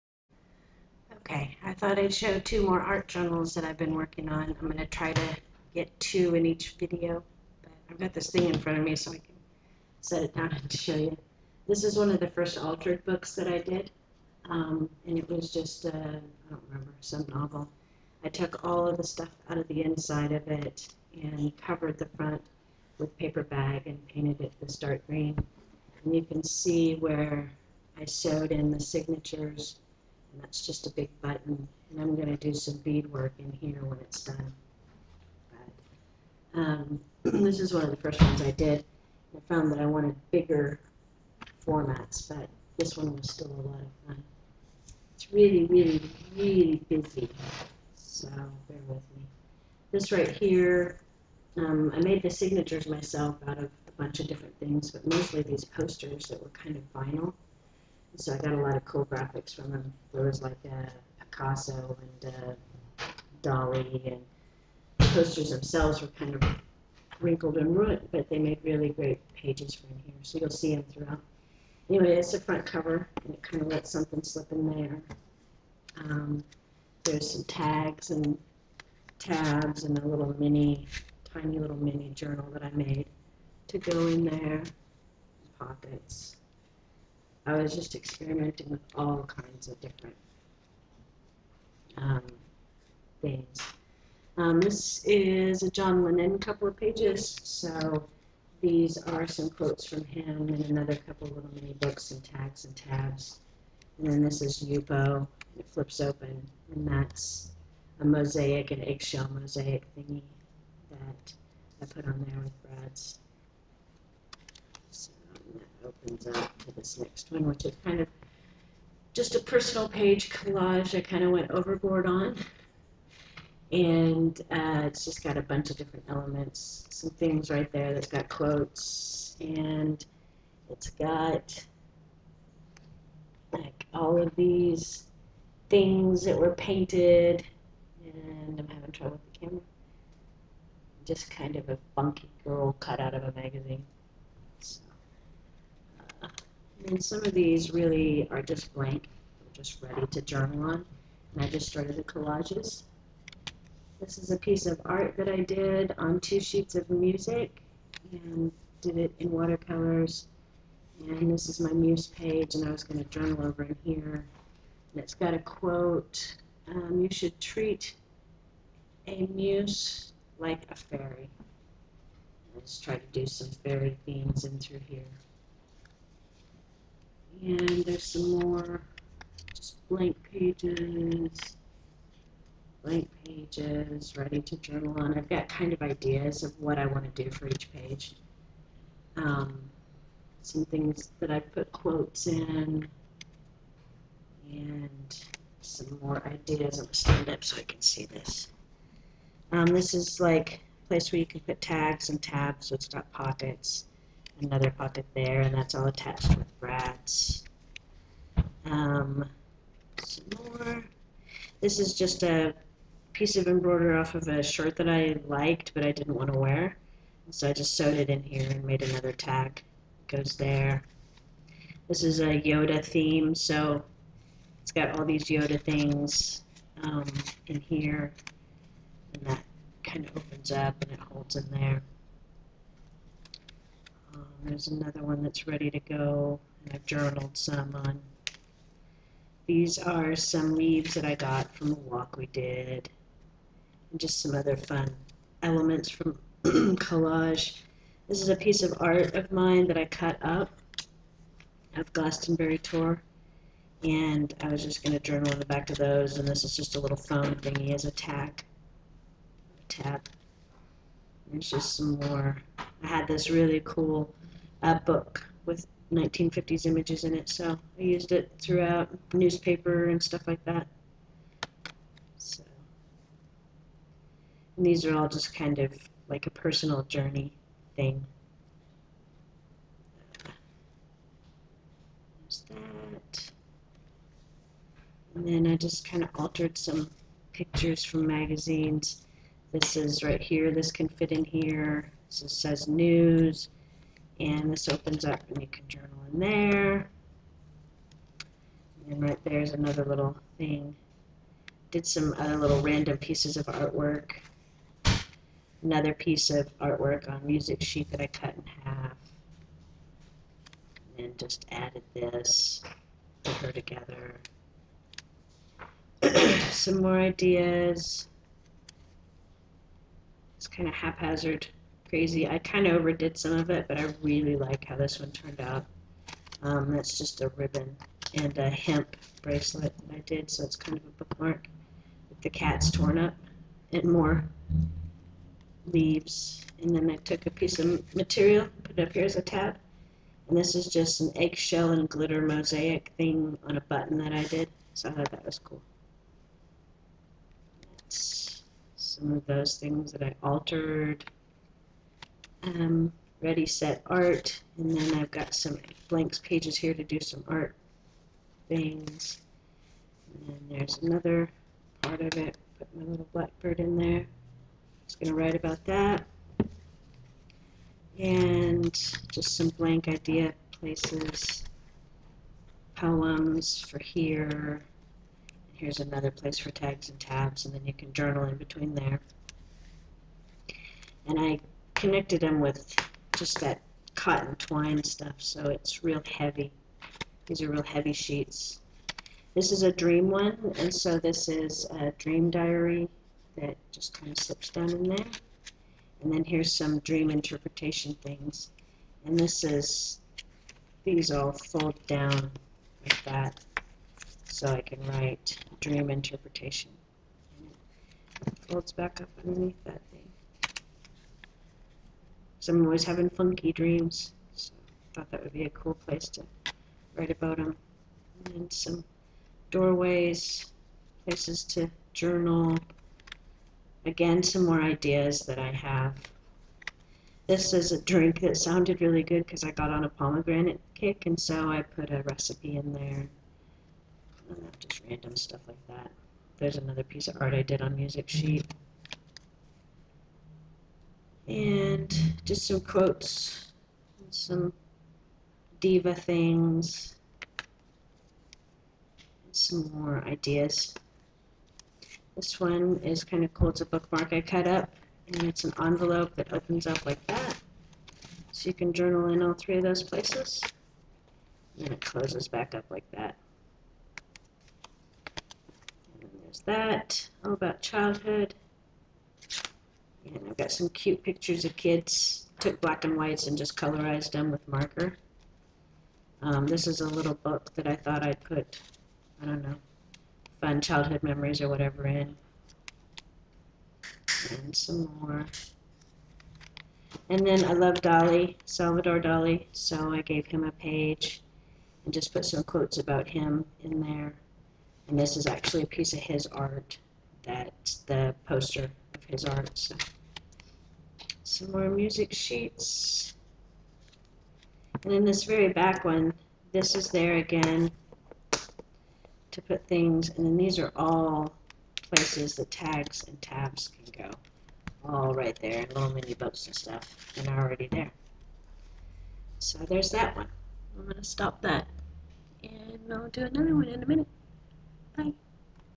Art Journal flip through - altered books, small format
This is a flip through of a work in progress art journal made from an altered book where I experimented with several techniques